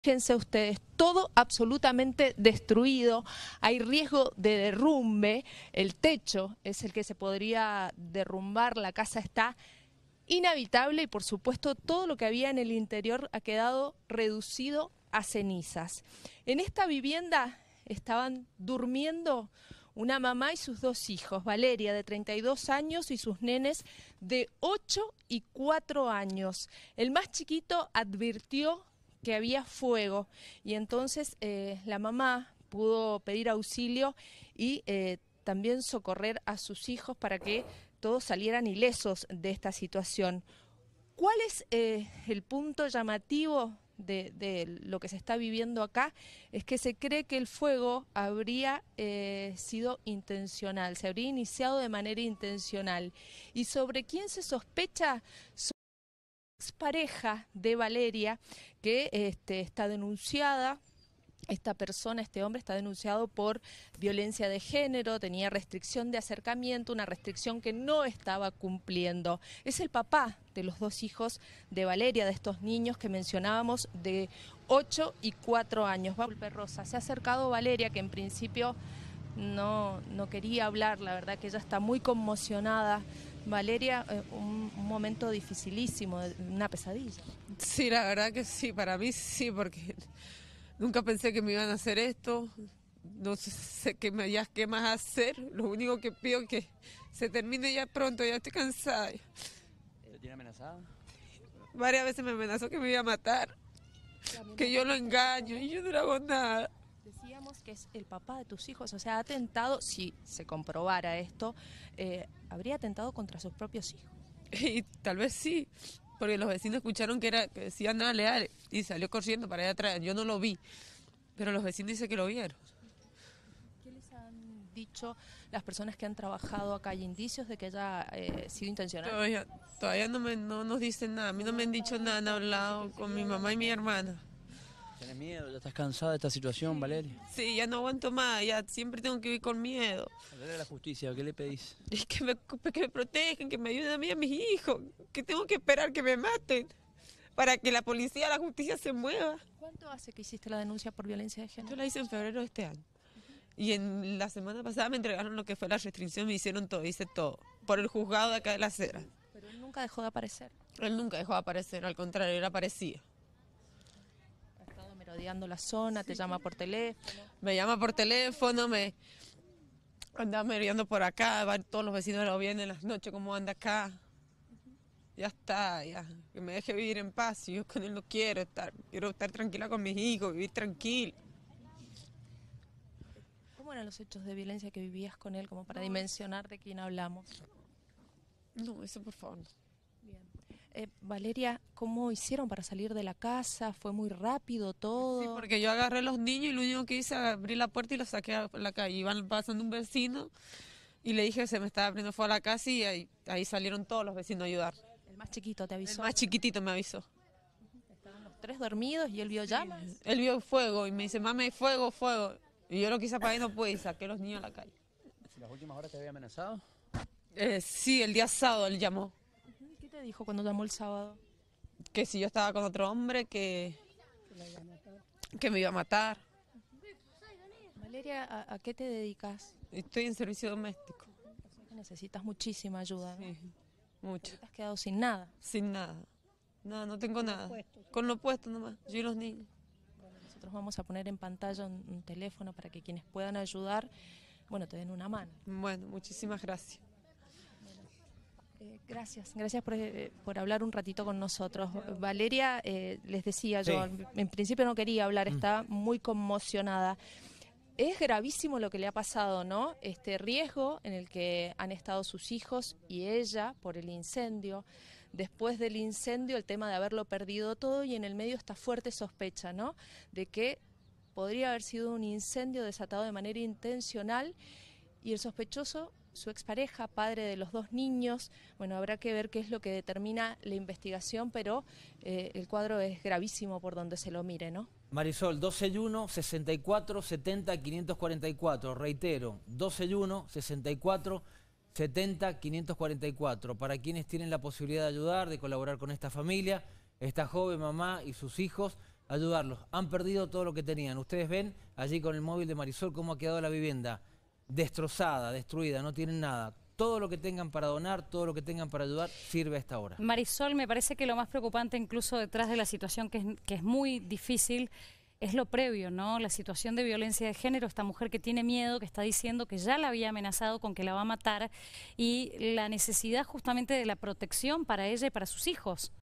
😌Se INCENDIÓ una CASA en LAS HERAS
Durante la mañana de este martes se incendió una casa en Las Heras. En la vivienda se encontraba una mujer junto a sus dos hijos. La víctima sospecha que el autor del hecho fue su ex pareja, padre de los menores, quien ya está denunciado por violencia de género.